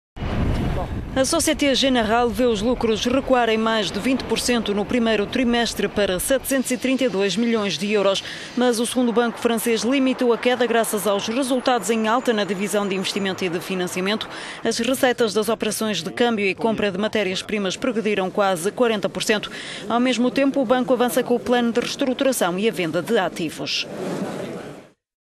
Société Générale regista forte queda dos lucros
No primeiro trimestre, a Société Générale viu os lucros recuarem mais de 20% para 732 milhões de euros.

Mas o segundo banco francês limitou a queda graças aos resultados em alta na divisão de investimento e de financiamento. As receitas das operações de câmbio e compra de matérias-primas progrediram quase 40 por cento. Ao mesmo tempo, o banco avança com o plano de reestruturação e a venda de ativos.